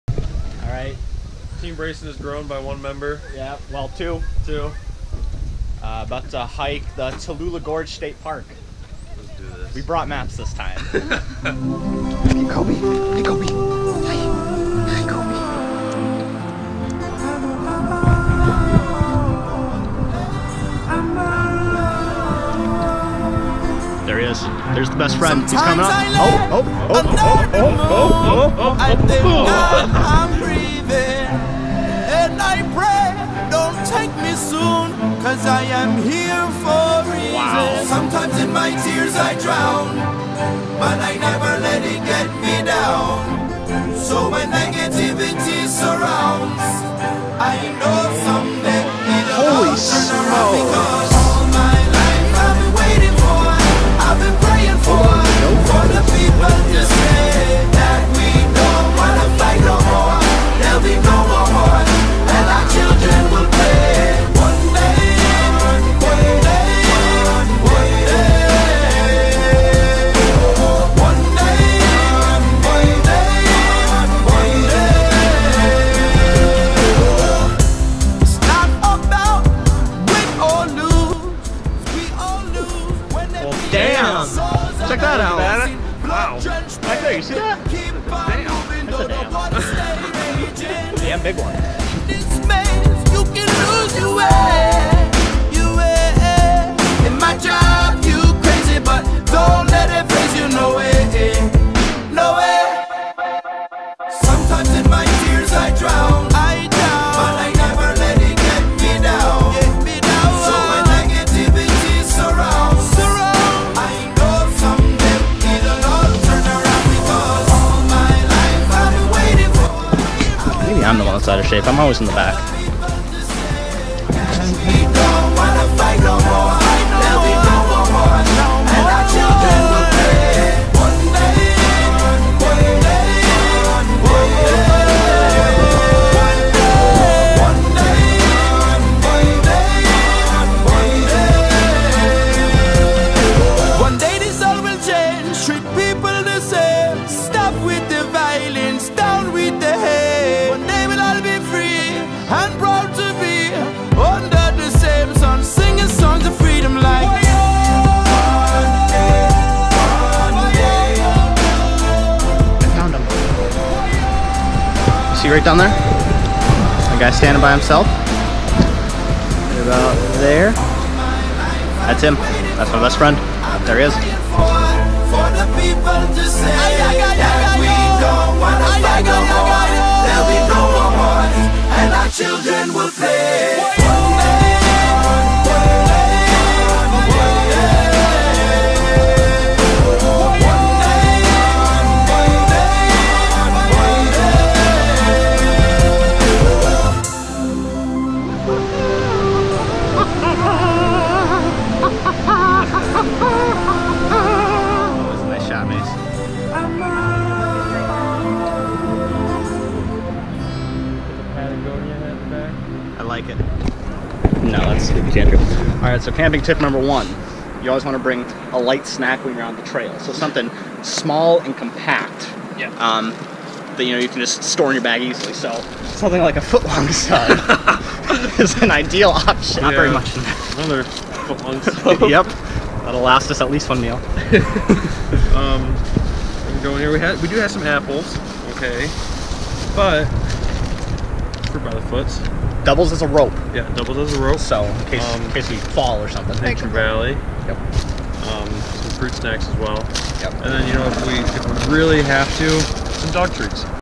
Tallulah Gorge State Park
Met up with Mason, Debora and Kobe Buckets Bryant this weekend to go for a quick hike at Tallulah Gorge State Park in Clayton, Georgia!